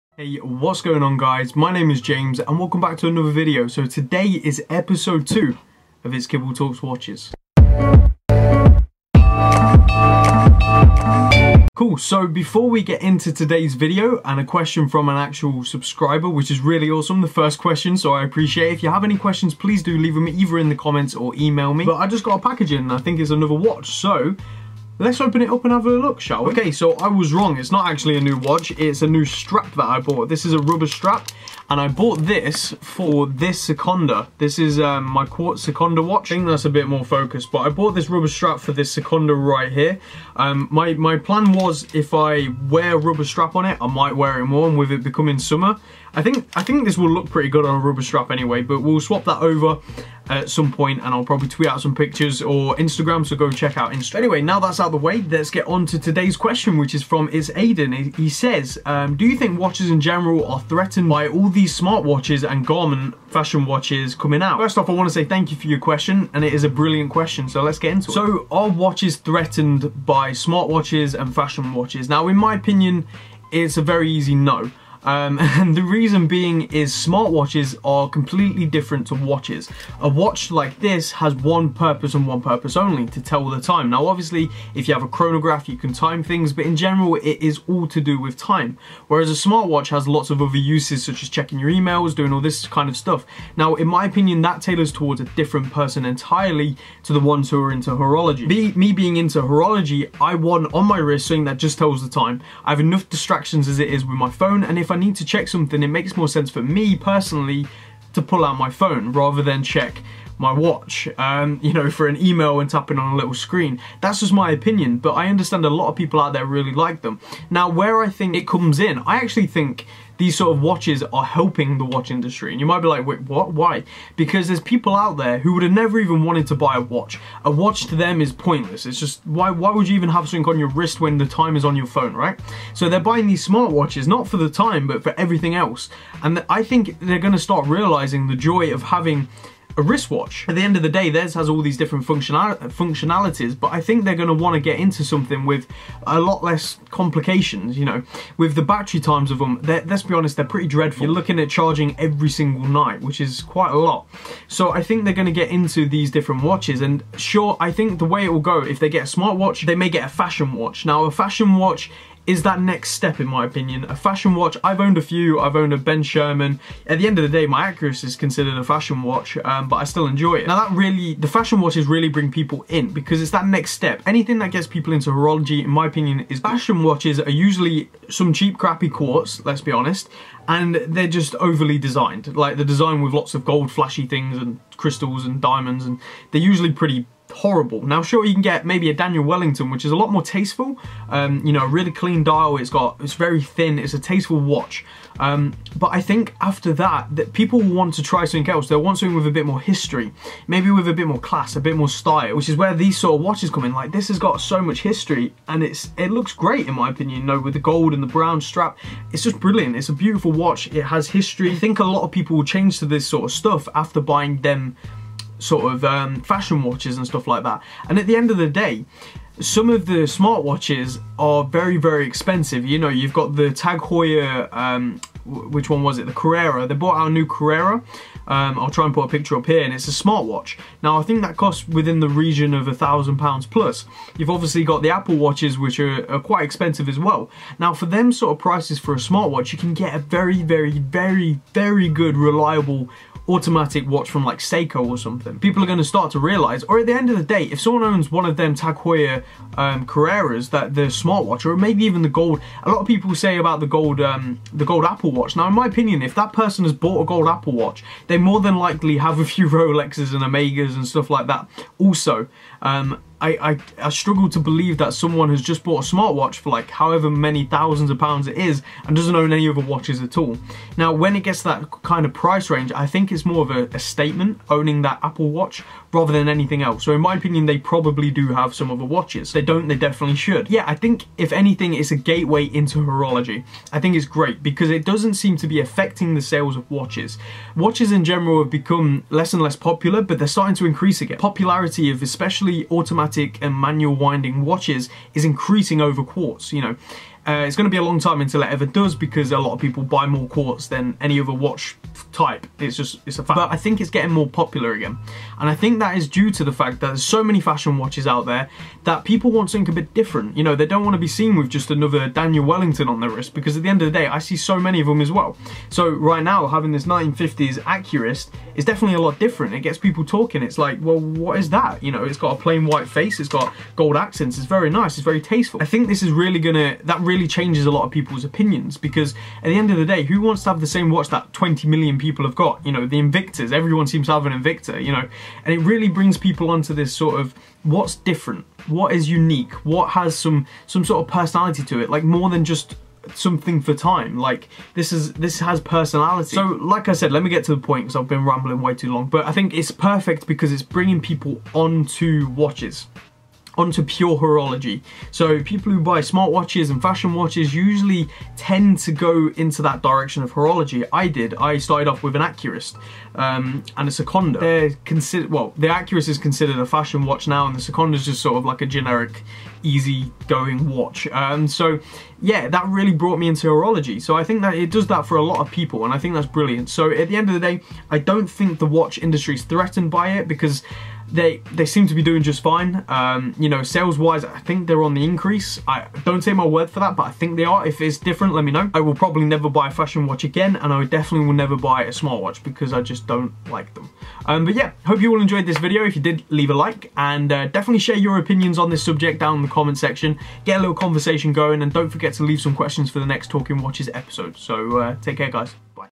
IKTW | Smart & Fashion Watches | Episode 2
Welcome to Episode 2 of IKTW (itsKibble Talks Watches)! In this episode we discuss "Smart Watches & Fashion Watches" and whether "They have an Impact on the Watch Industry"!